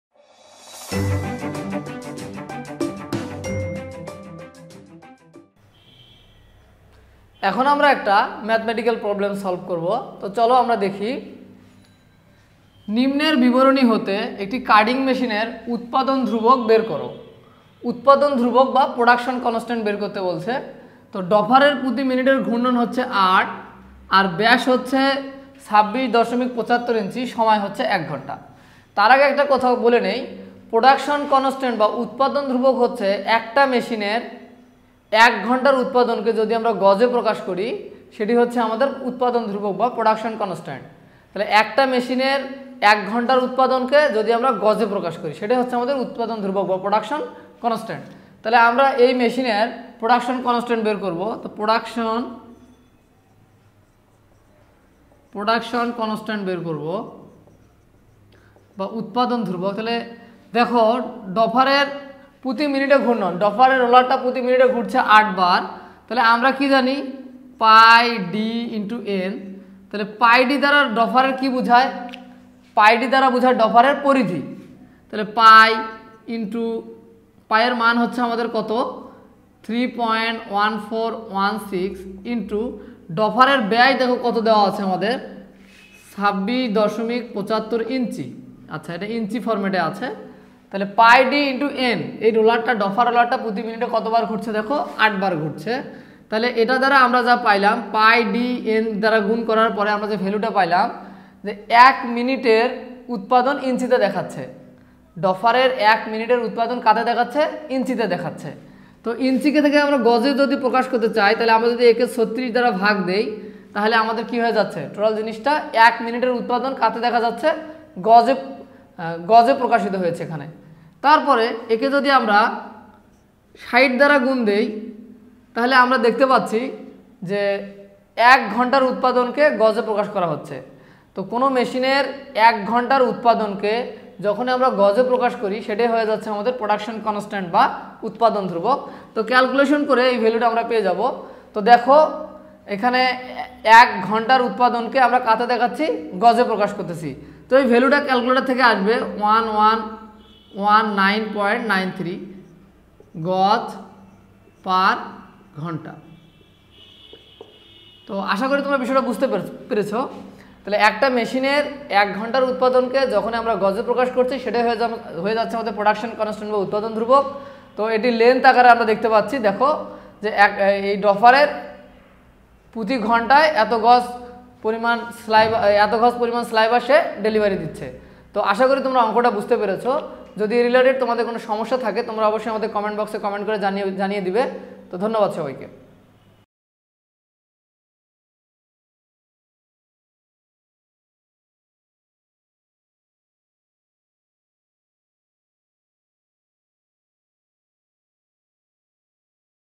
Carding Machine Part - 09 । কার্ডিং মেশিন । টেক্সটাইল ক্যালকুলেশন । Textile Calculation - 01
বিষয় :  Carding Machine Part -09 । কার্ডিং মেশিন । টেক্সটাইল ক্যালকুলেশন । Textile Calculation -01
 ইন্সট্রাক্টর : মোঃ রেজাউল করিম সবুজ
              (বি.এসসি ইন টেক্সটাইল ইঞ্জিনিয়ারিং, ডুয়েট)